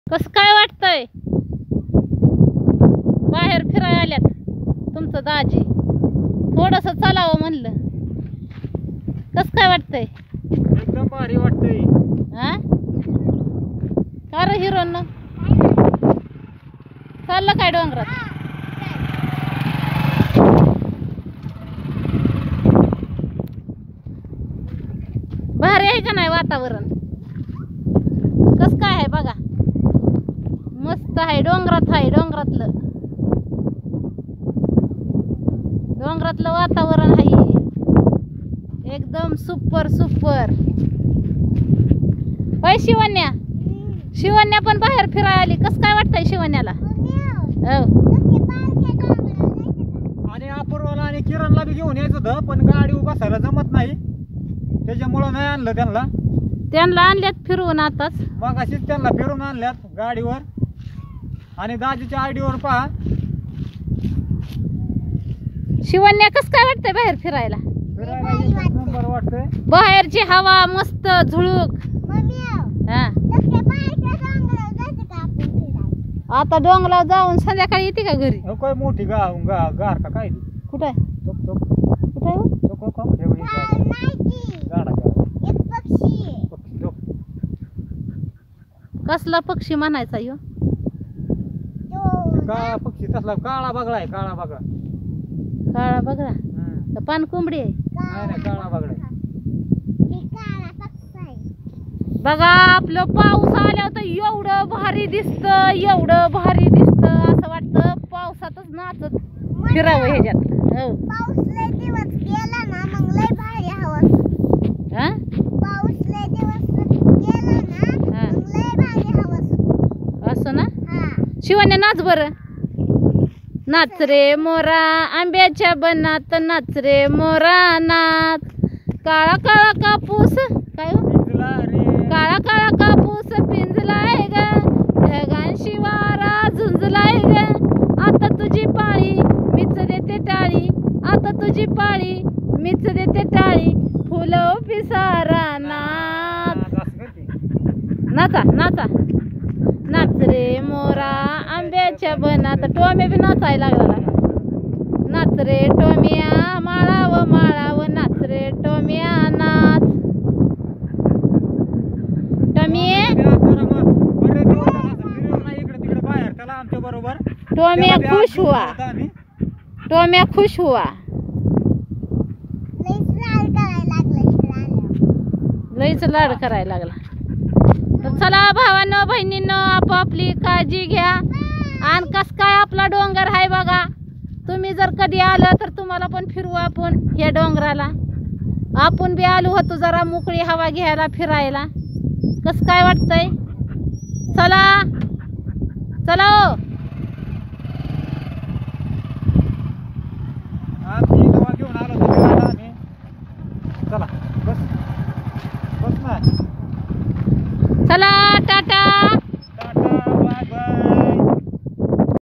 Poonamtupe घरात बसून बसून कोंडल्यावाणी झालतं आल तू जरा बाहेर फिरायला डोंगरात🤔
# पुनम तुपे आमचं भगुलं हाय#